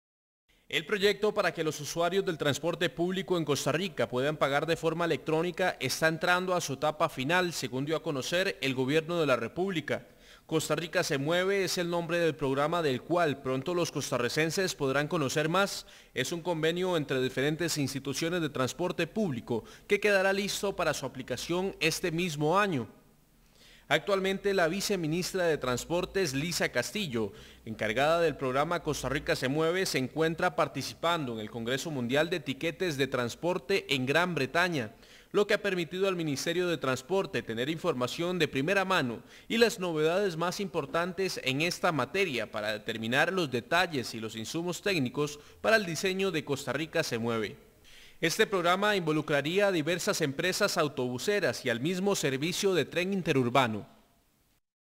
Costa Rica anuncia que en 2017 iniciará a operar pago electrónico en sistemas de transporte
Tras varios meses de atraso, Costa Rica podrá contar pronto con la necesaria modernización del transporte público que permitirá tener la institucionalidad y la seguridad jurídica para definir el modelo de pago electrónico.

Juan Diego Córdoba / CB24